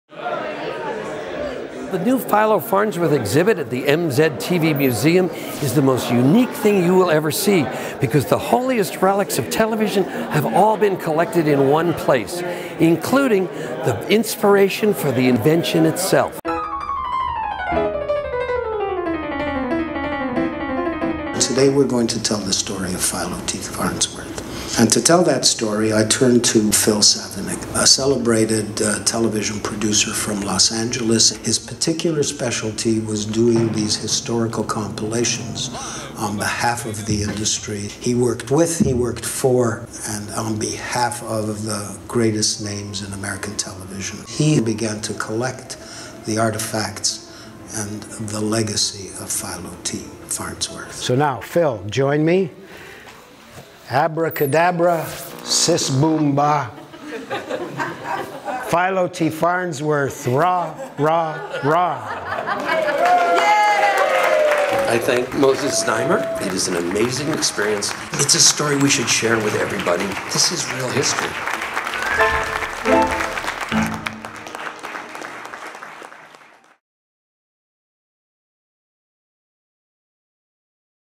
PhiloT.Farnsworth, Forgotten Genius: The Boy Who Invented Electronic TV
Take a look at this fantastic article from The Toronto Star about our newest exhibit, "Philo Farnsworth, Forgotten Genius: The Boy Who Invented Electronic Television" at Moses Znaimer's MZTV Museum of Television ➢

Let's flash back to the official launch, shall we? Curated by Phil Savenick of Los Angeles, California, the exhibit tells the story of the father of electronic television, Dr. Philo T. Farnsworth, and his journey to bring his grand idea to fruition.

As a young boy, he was inspired by 20th century sciencefiction stories, and often gazed at the moon, while reflecting on his fantasies of the future of technology. And it was on September 7th, 1927, that Philo Farnsworth transmitted the very first electronic image. The young inventor was delighted, and later in 1934 demonstrated television to the public for the first time!

STILL ON, FOR A LIMITED TIME!

Don’t miss this epic exhibit at the MZTV Museum of Television. You won’t regret it!